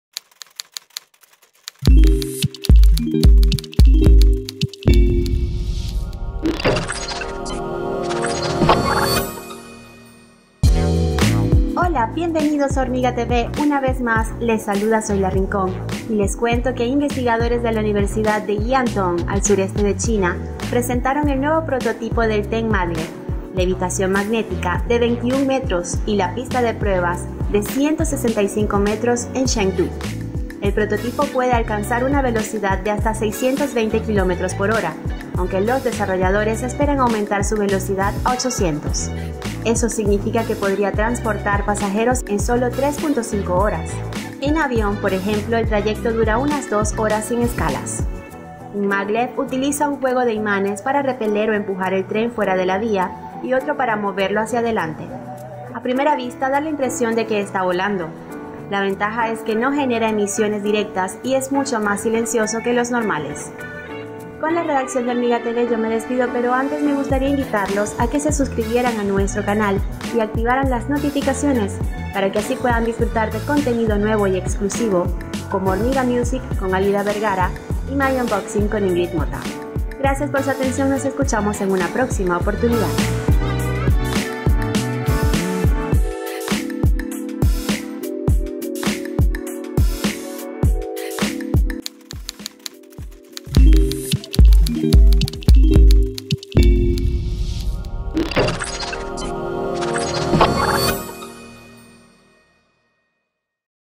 CHINA APUESTA A TREN DE LEVITACIÓN MAGNÉTICA ULTRARRÁPIDO | TRANSPORTE | HormigaTV
Un prototipo de tren que utiliza la tecnología de levitación magnética superconductora de alta temperatura fue lanzado en la ciudad de Chengdu, en el suroeste de China. El tren maglev de desarrollo local cuenta con una velocidad diseñada de 620 kilómetros por hora, según la Universidad Jiaotong del Suroeste, uno de los participantes en su diseño. En un vídeo subido a YouTube por South China Morning Post se muestra el nuevo prototipo y lo que podría ser el futuro de los viajes en trenes de alta velocidad sin emisiones en China.